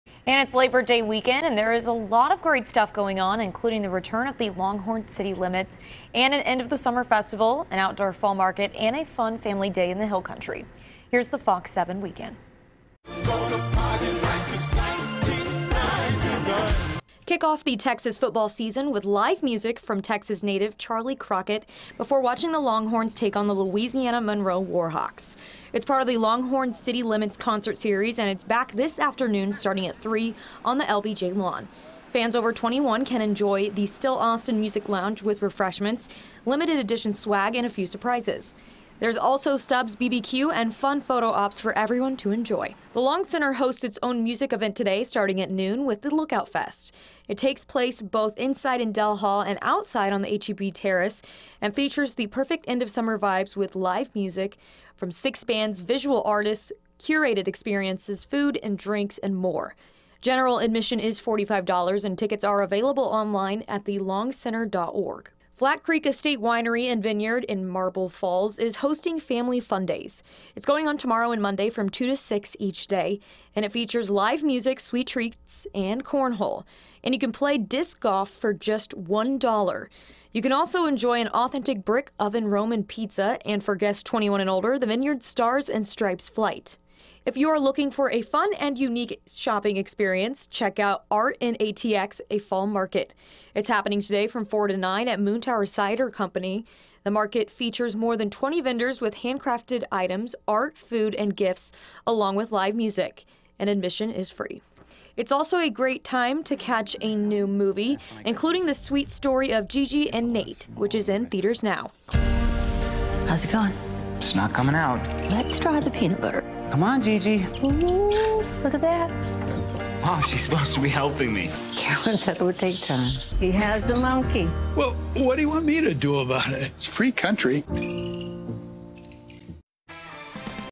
FOX 7 Weekend: Longhorn City Limits, Look Out Fest, Art in ATX, and more | FOX 7 Austin
Looking for something to do this weekend? Good Day Austin's Libbi Farrow shares some fun events happening, including Longhorn City Limits on the LBJ Lawn with Texas native Charley Crockett, the Look Out Fest at the Long Center, Family Fun Days at Flat Creek Estate Winery and Vineyard, the Art In ATX: A Fall Market at Moontower Cider Company and a new movie in theaters called "Gigi and Nate".